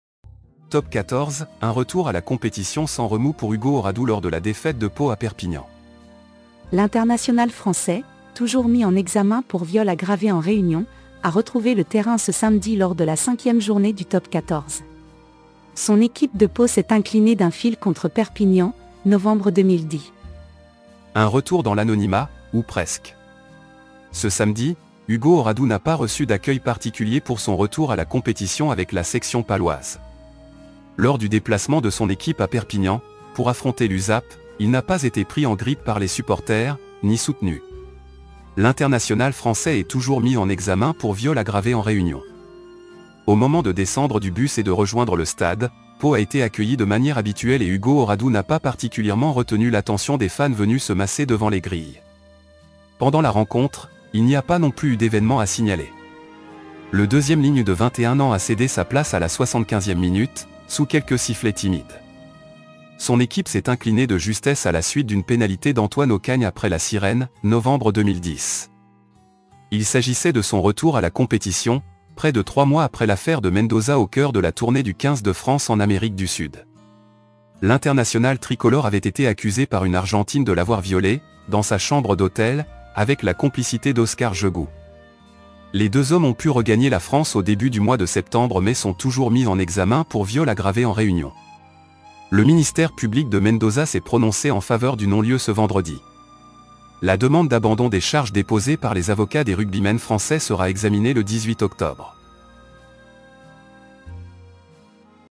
Top 14 : un retour à la compétition sans remous pour Hugo Auradou lors de la défaite de Pau à Perp..
Top 14 : un retour à la compétition sans remous pour Hugo Auradou lors de la défaite de Pau à Perpignan

L’international français, toujours mis en examen pour viol aggravé en réunion, a retrouvé le terrain ce samedi lors de la cinquième journée du Top 14. Son équipe de Pau s’est inclinée d’un fil contre Perpignan (11-10).